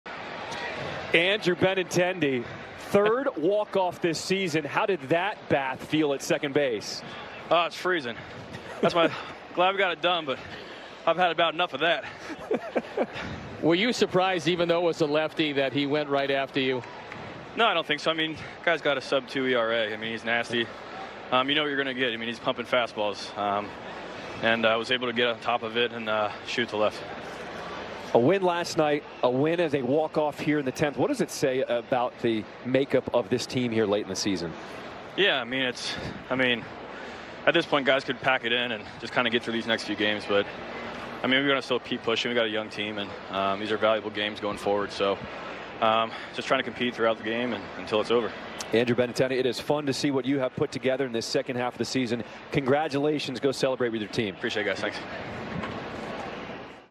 Andrew Benintendi Postgame Interview after Hitting 10th Inning Walk Off vs Angels!
Andrew Benintendi postgame interview from Los Angeles Angels vs Chicago White Sox | 9/25/24 | 2024 MLB Season